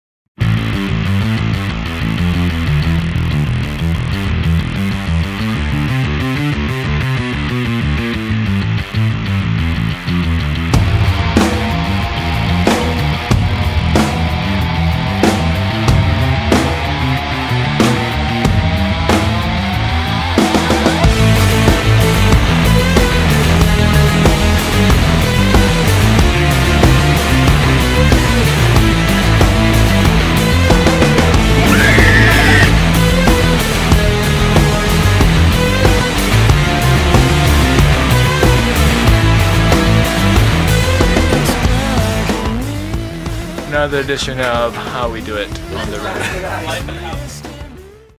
Loading Up
Bread of Stone loading up their trailer after a show.